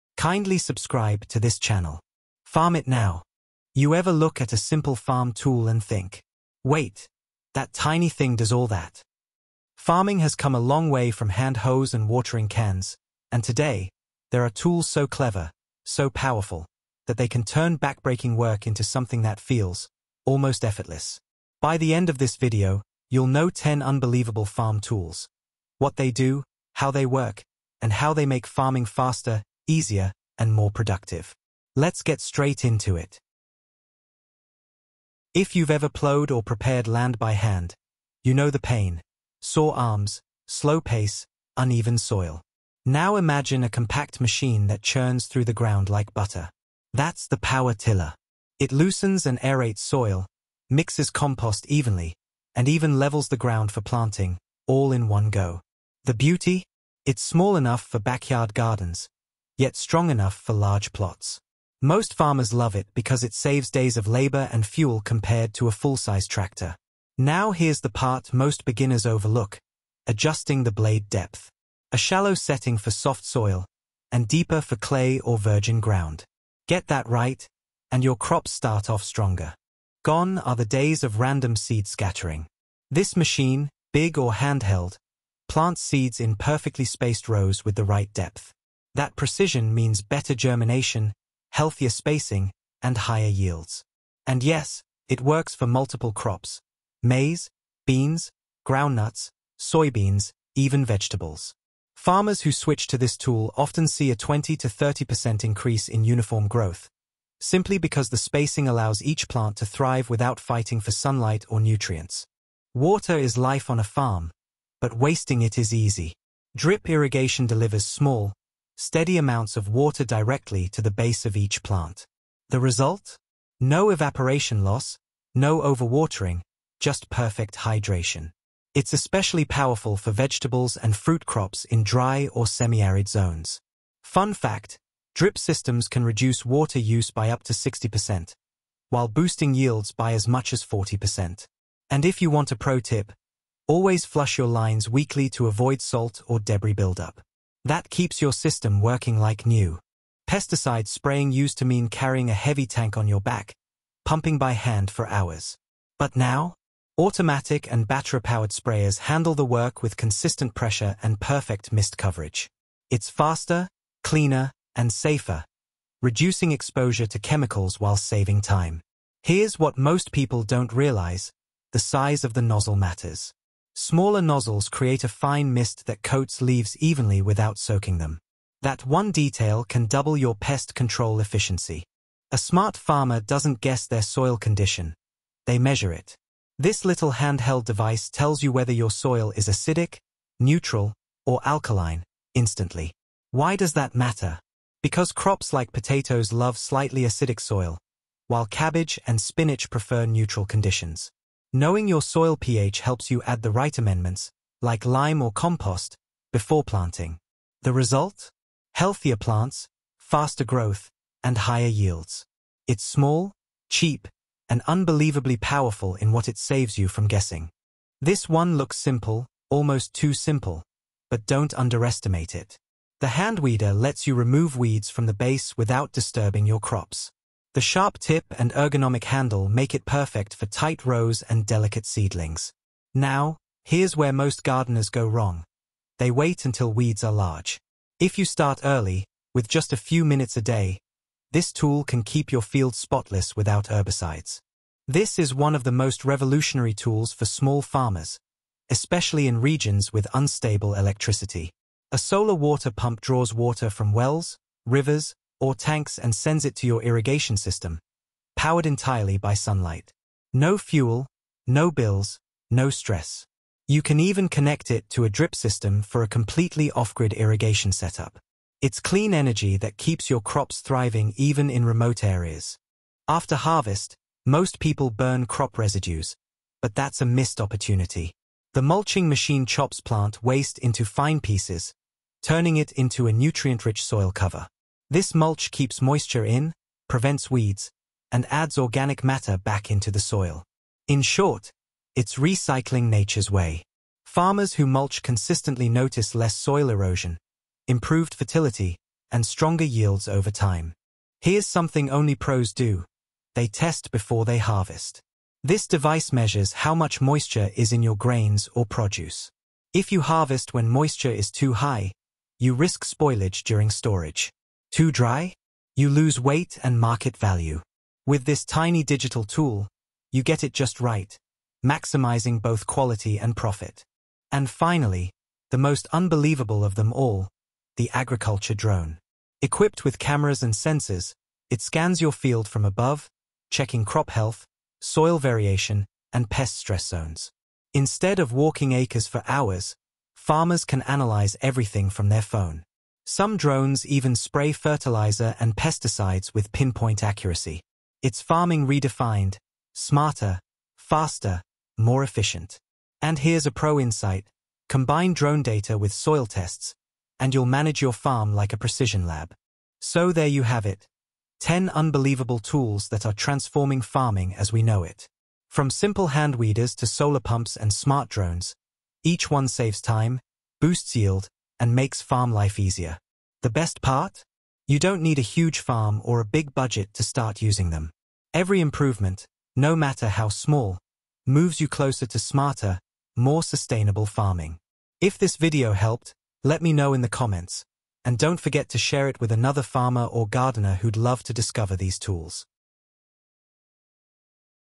10 Unbelievable Farm Tools and Their Uses | Sustainable Agriculture
Discover the most unbelievable farm tools that are transforming modern agriculture! From hand weeders to smart drones, this video explores 10 essential farming tools and their incredible uses that help farmers save time, boost yield, and reduce labor costs. Whether you’re a small-scale gardener or a commercial farmer, you’ll find practical insights and pro tips for every tool.

We’ll cover tools like power tillers, solar water pumps, drip irrigation systems, and more — all designed to make your farming easier and more efficient.

By the end of this video, you’ll understand how each of these farm tools works, why they matter, and how to use them effectively for maximum productivity.